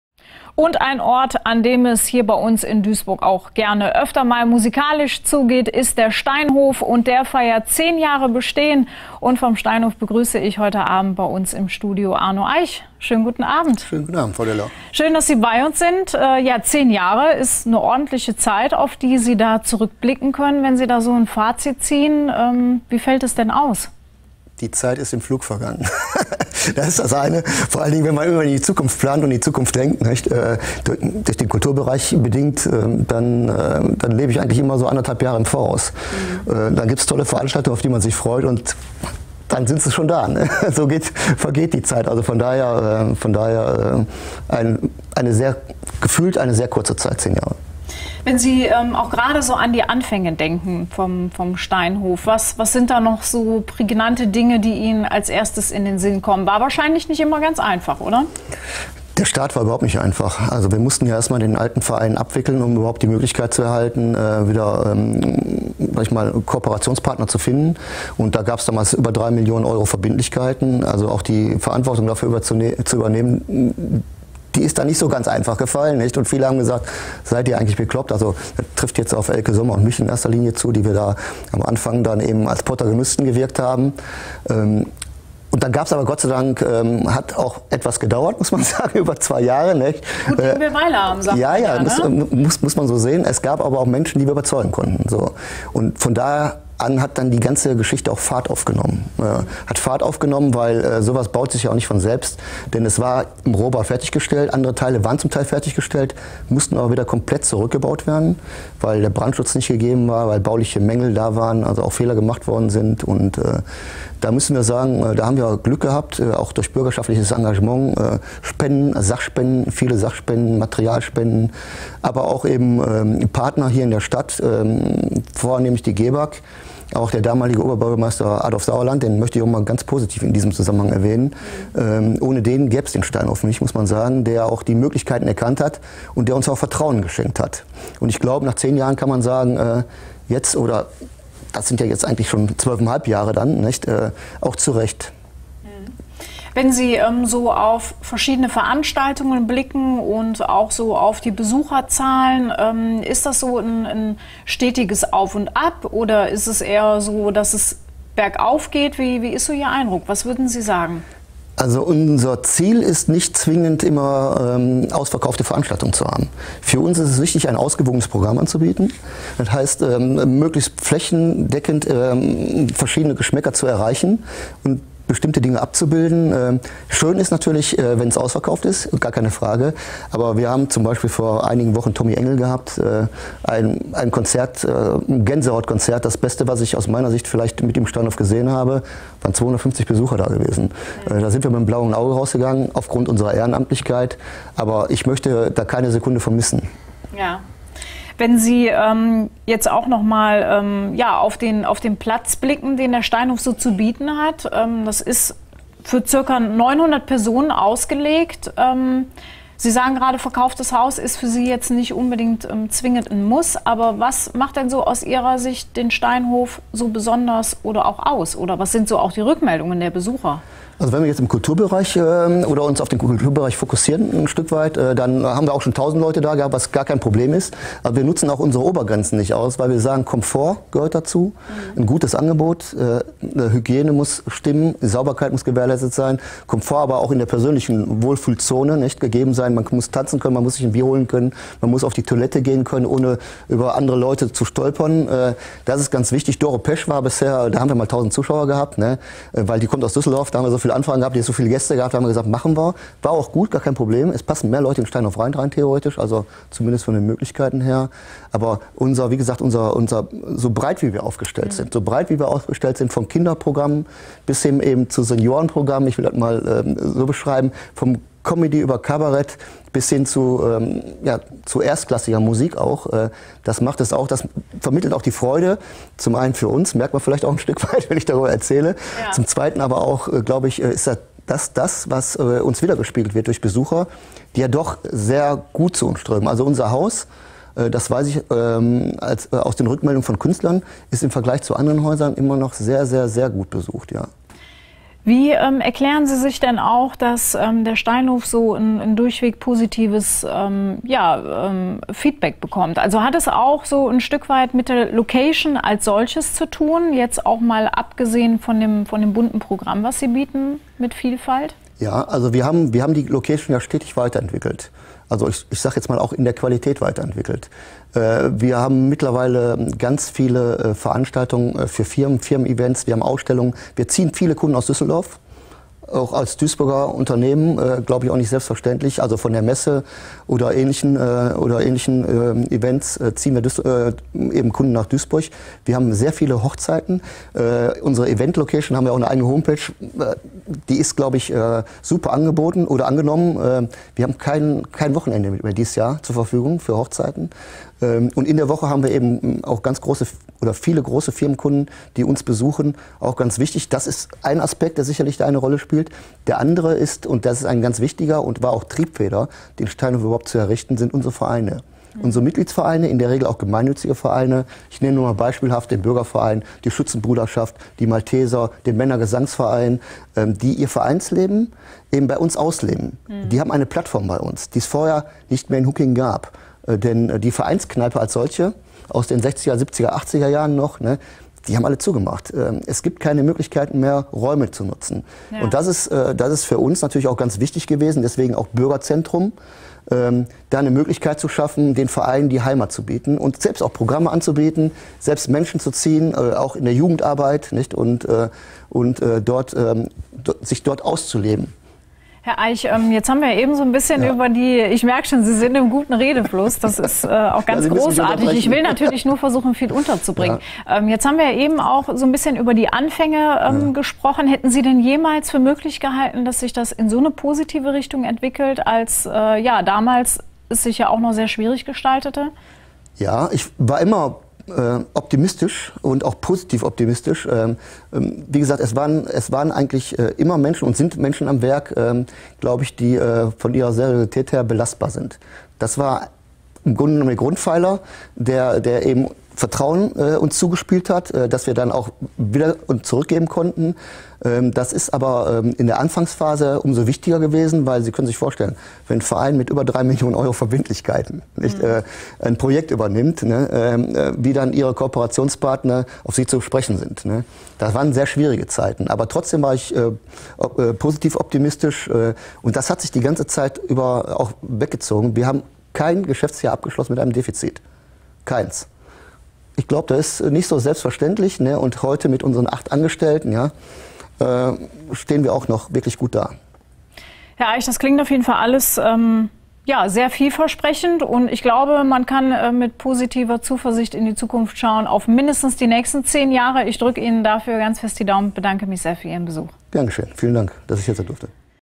STUDIO 47 .live | ARNO EICH ÜBER 10 JAHRE STEINHOF DUISBURG
Im Interview spricht der 1. Vorsitzende Arno Eich über das 10-jährige Jubiläum des Steinhof Duisburg.